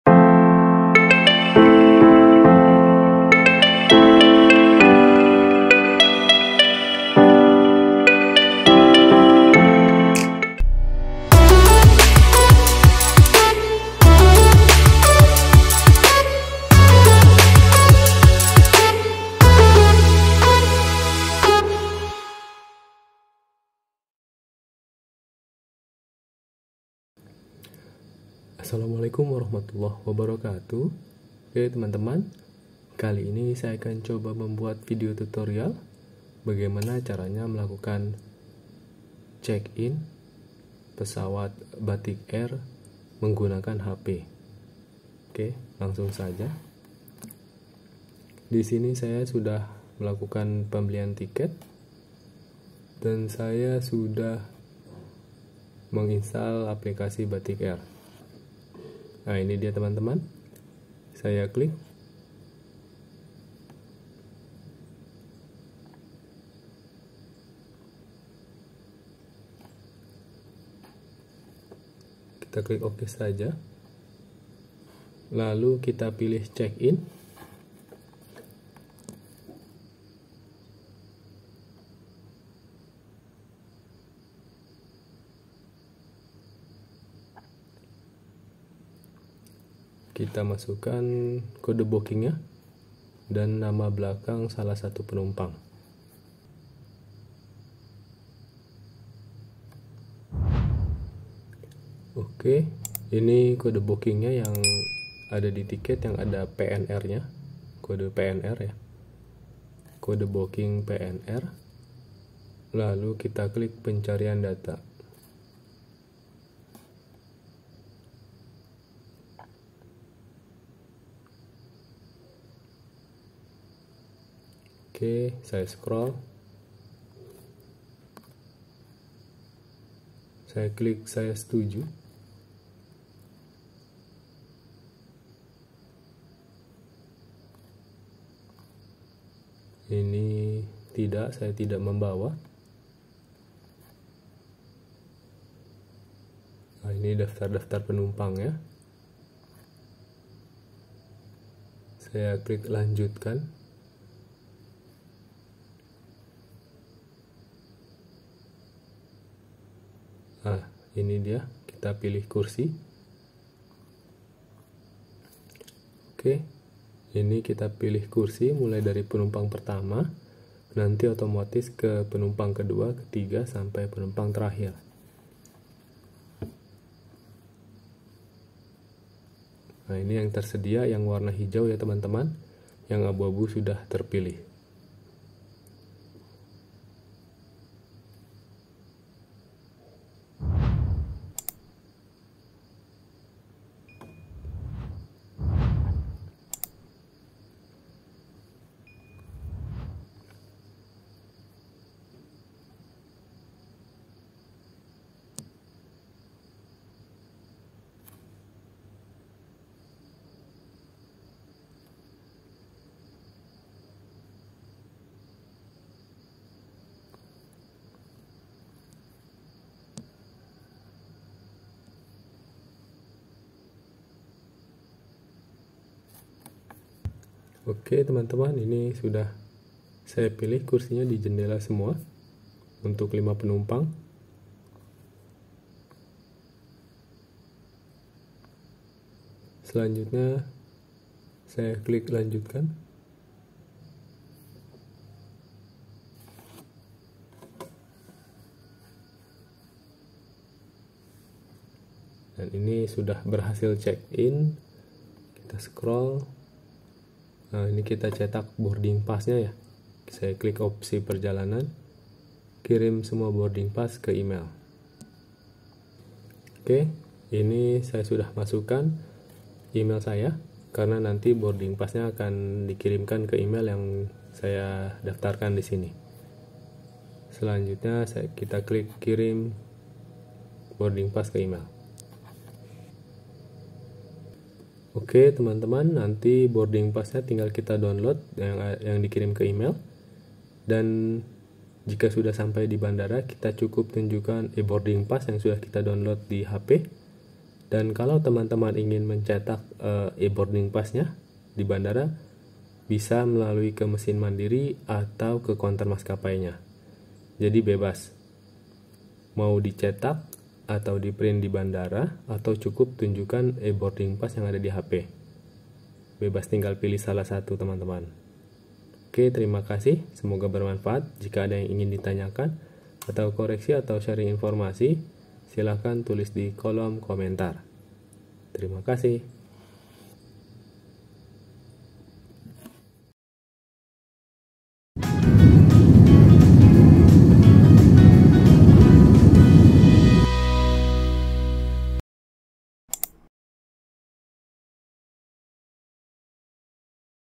PANDUAN CHECK IN DAN CETAK BOARDING ONLINE PESAWAT BATIK AIR BISA PILIH KURSI
PANDUAN CHECK IN DAN CETAK BOARDING ONLINE PESAWAT BATIK AIR BISA PILIH KURSI, Cuma pake HP bisa dilakukan dimanapun hanya 5 menitan dan saat di bandara tidak perlu lagi antri ke counter maskapainya

By :

Dzikri Channel